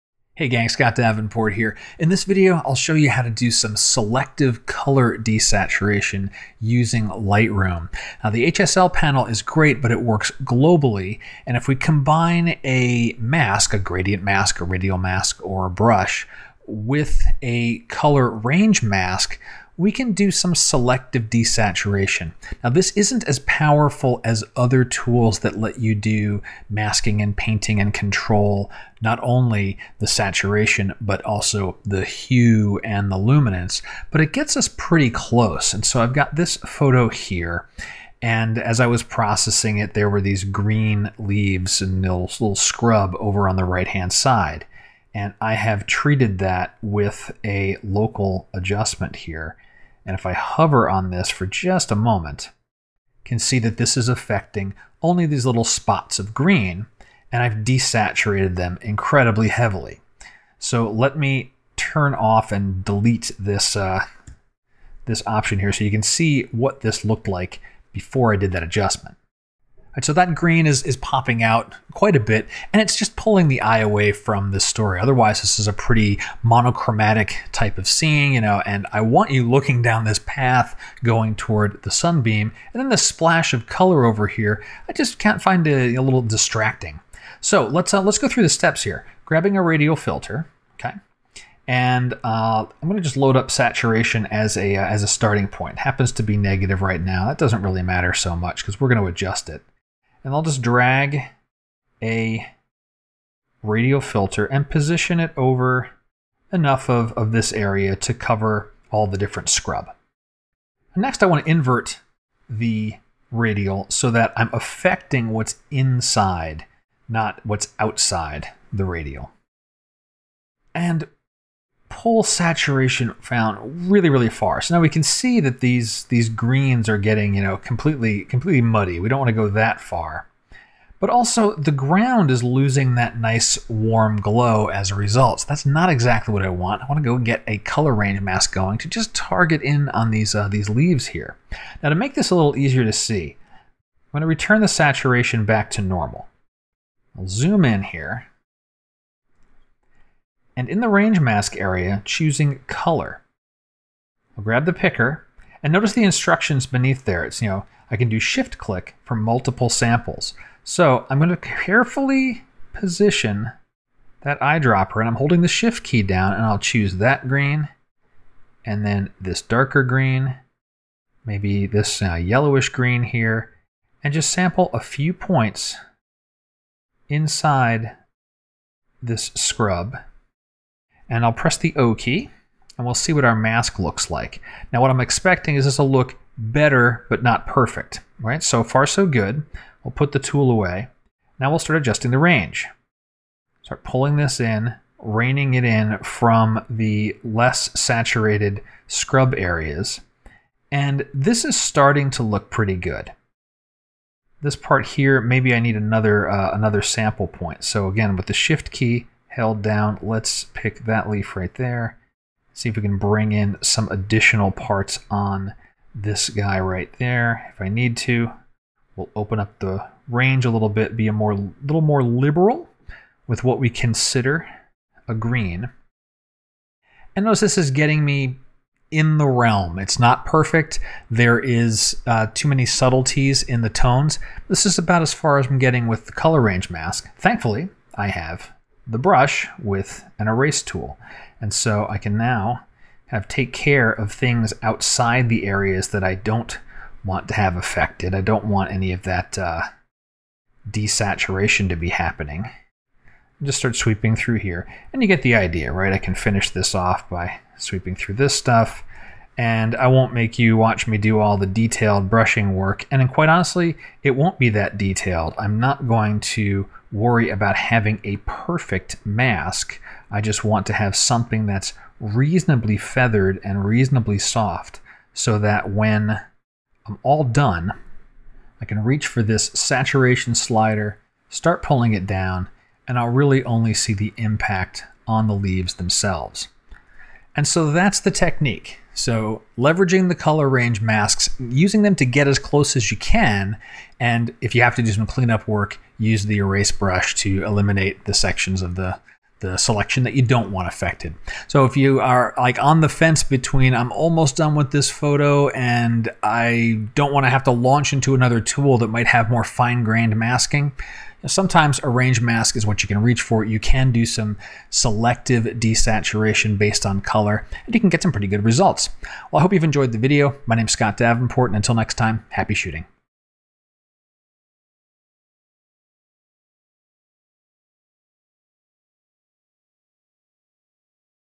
How To Use Range Masks To Selectively Apply Saturation In Lightroom
The HSL panel in Lightroom works globally. Sometimes, you want to adjust colors in segments of your photo. One option is to use a localized gradient or radial filter in combination with a color range mask. This technique came in handy for me very recently while processing a photo from Big Sur.